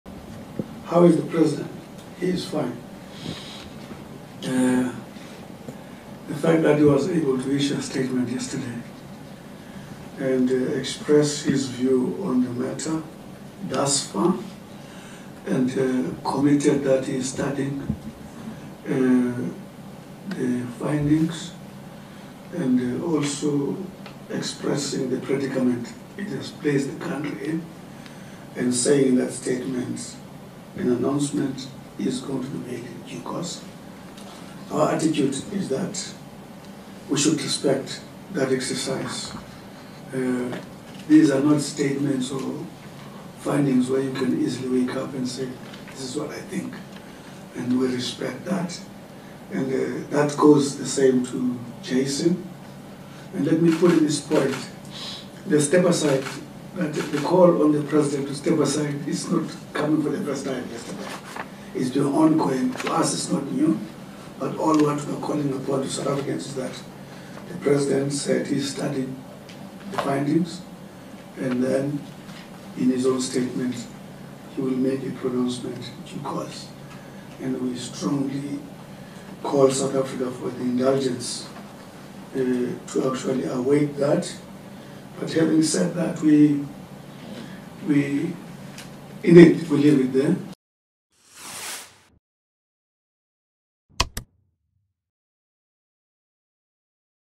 WATCH | Phala Phala fallout: Ramaphosa will make announcement in due course - Gungubele
In light of the Phala Phala matter, Minister in the Presidency Mondli Gungubele says President Cyril Ramaphosa is fine. Gungugbele addressed a press briefing in Cape Town on Thursday 1 December.